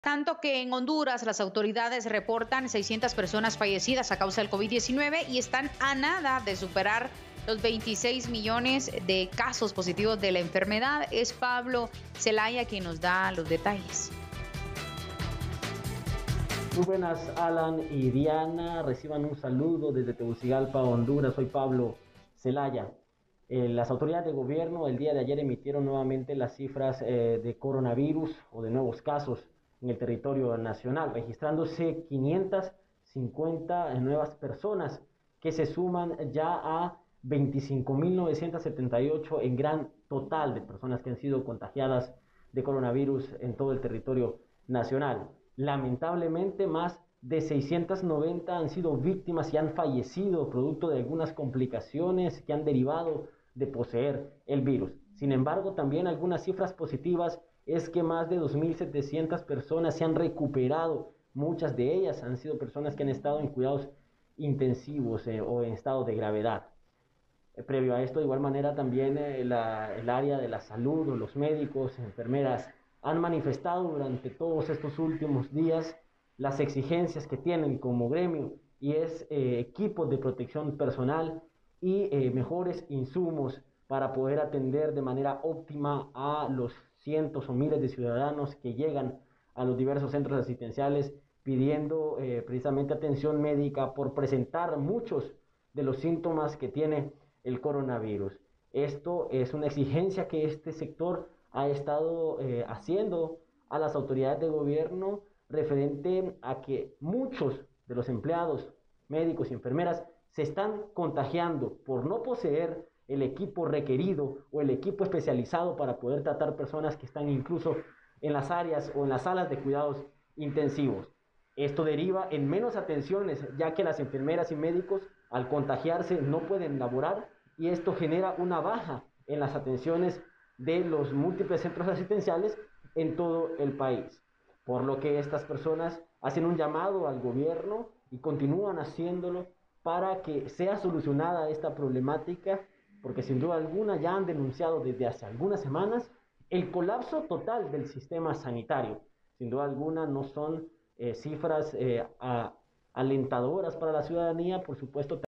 600 muertes por COVID-19 en Honduras
Más de 2.700 personas se han recuperado

El periodista Pablo Zelaya nos tienes los detalles de la situación por el COVID-19 en Honduras. Visita nuestro sitio